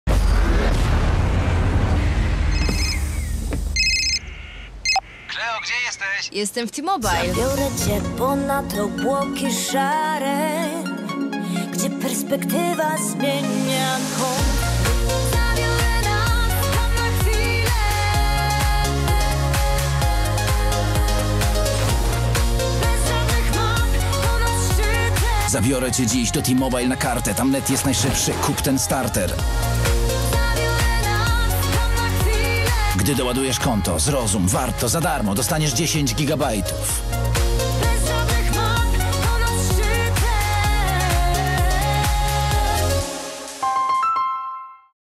Hit od T-Mobile! 10 GB za darmo po doładowaniu!
Tego jeszcze nie było - 10 GB najlepszego internetu za darmo po doładowaniu w T-Mobile na Kartę!